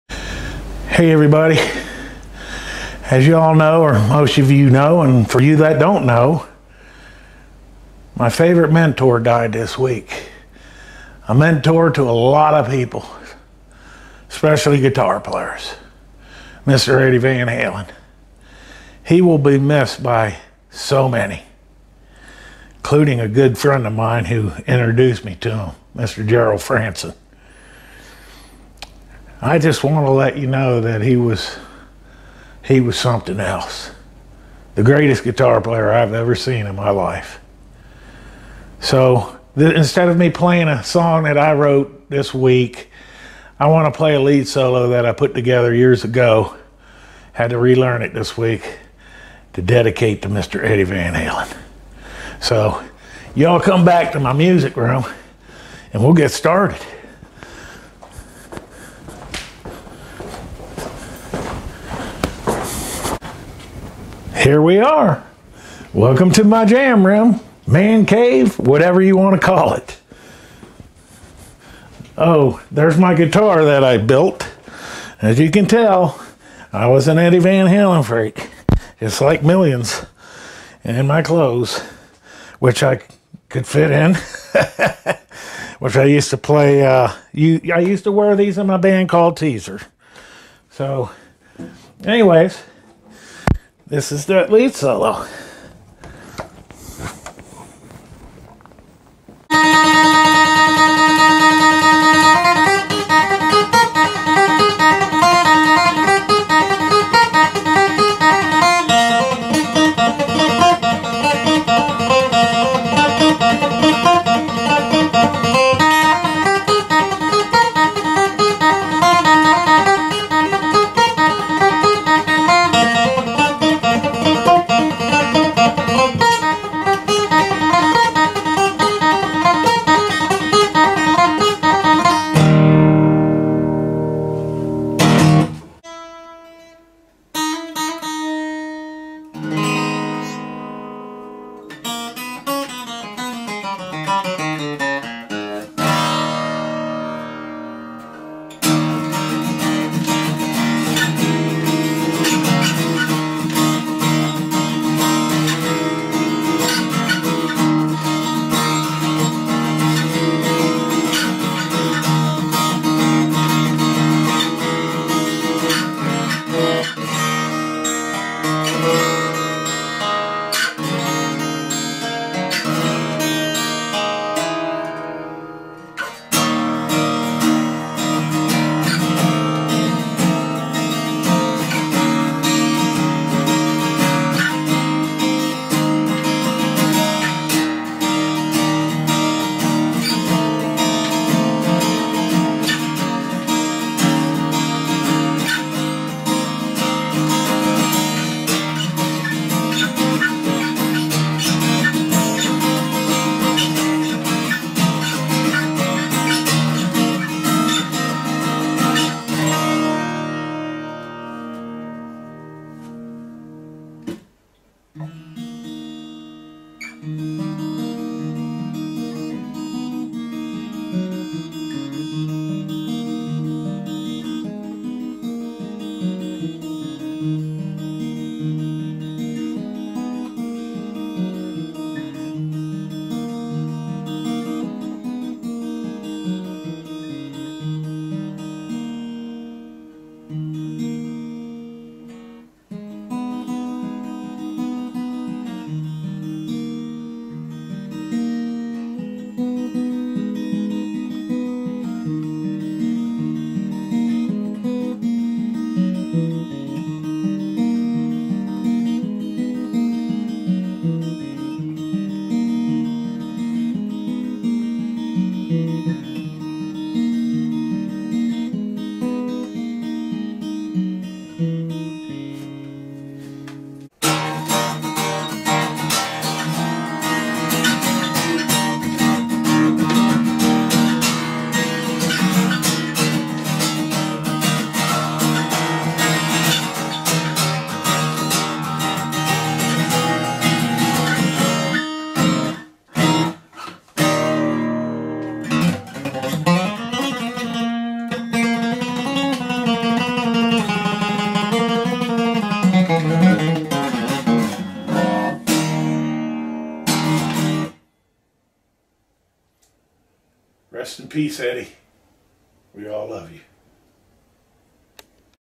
EDDIE Van Halen tribute lead solo...
This is a solo I put together years ago and I want to dedicate it to the memory of Eddie Van Halen. This week's song is all about the greatest guitar player I've ever known to walk this earth. Thank you for listening and please, if you like this solo. Thank you and God bless!
We all Love You Eddie!!